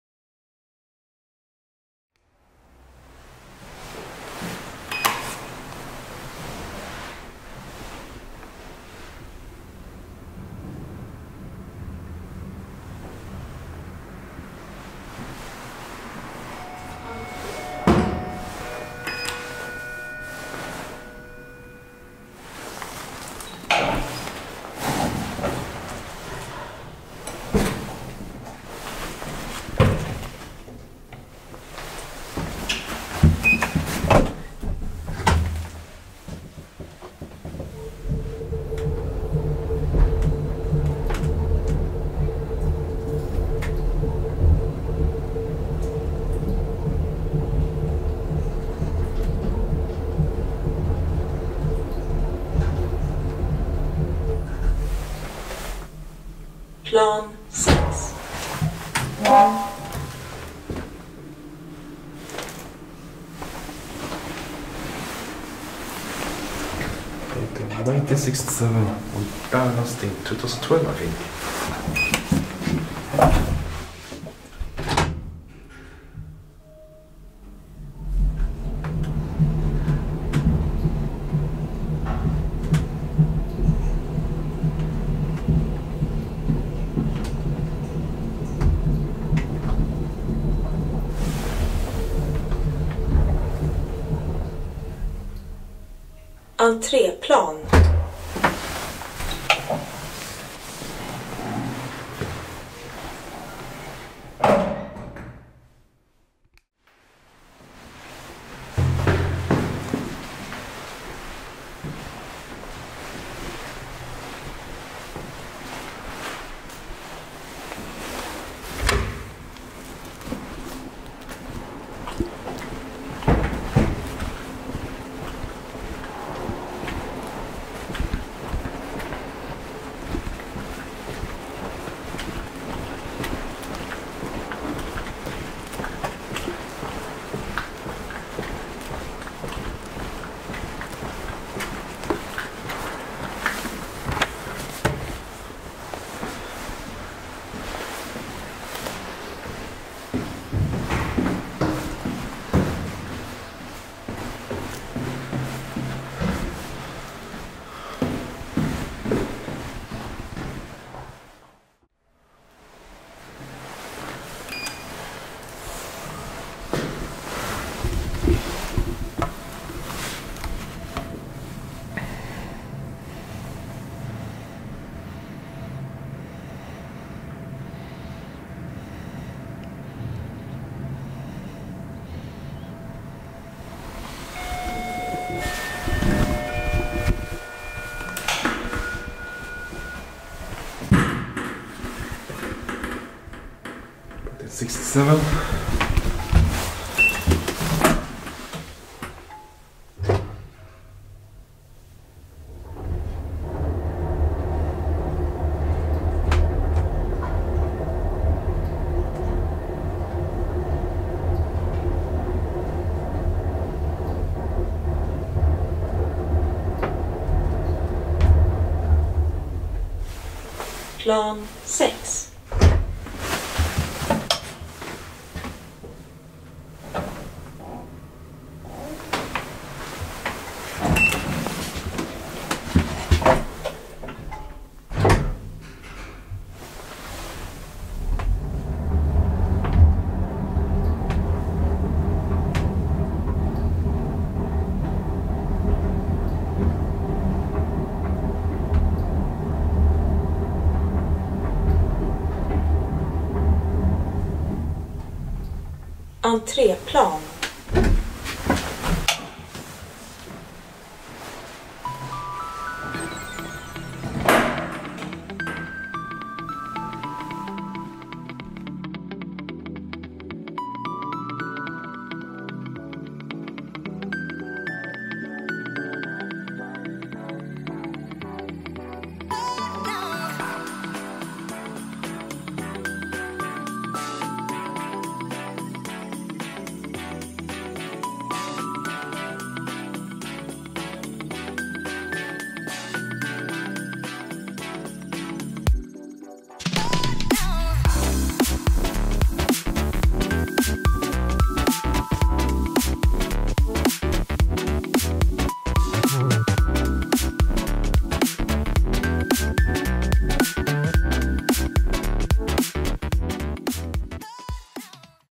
1967 KONE (MB: Amsler) Traction Elevators @ Timmermansgatan 29-31, Stockholm, Sweden
ELEVATOR INFORMATION!
Timmermansgatan 29 | KONE H 14702: 0:00
Walking from 29 to 31: 1:46
Timmermansgatan 31 | KONE H 14701: 2:41

Manufacturer/Tillverkare: KONE
Current modernizer/Senaste modernisering av: Amsler Hiss AB (Hisstema NP-1)
Serial Number/Tillverkningsnummer: H 14702 & H 14701
Capacity/Maxvikt: 480Kg - 6 Persons
Floors:/Antal våningar

Location/Adress: Timmermansgatan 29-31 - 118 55 Stockholm (Södermalm), Sweden